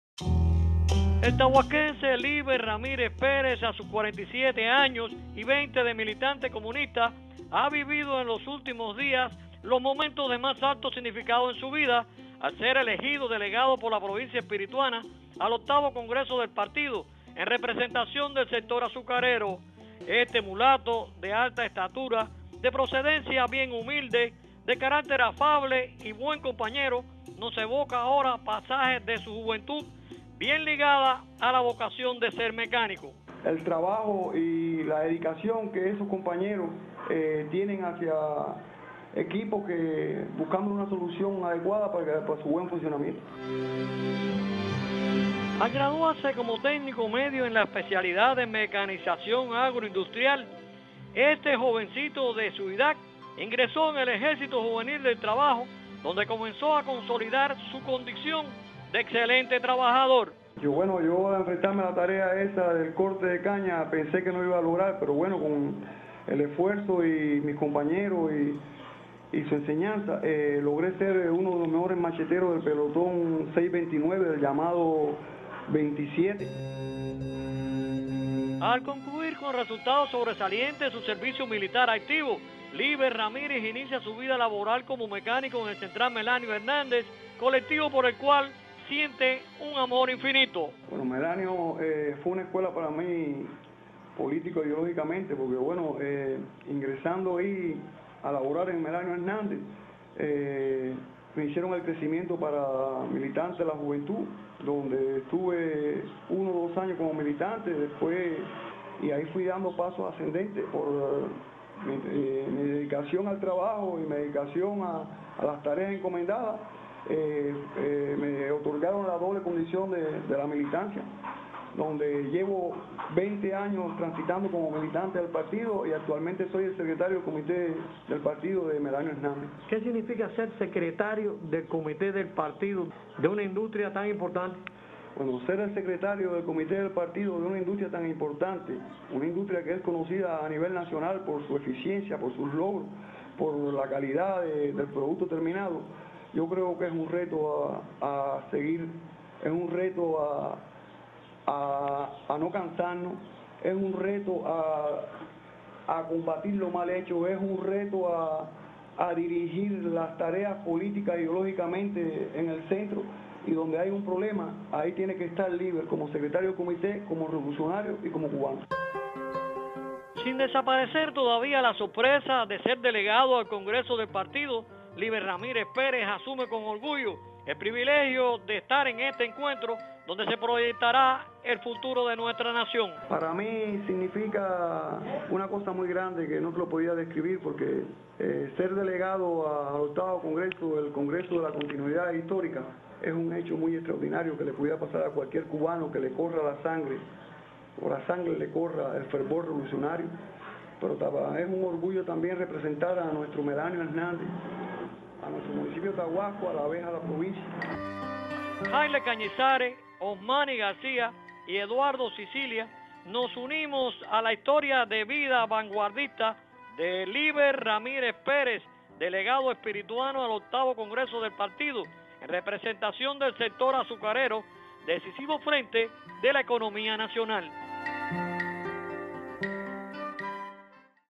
Líber Ramírez Pérez, uno de los integrantes de la delegación espirituana al Congreso del Partido
El taguasquense, Líber Ramírez Pérez es uno de los espirituanos que representará a la provincia en el Octavo Congreso del Partido Comunista de Cuba.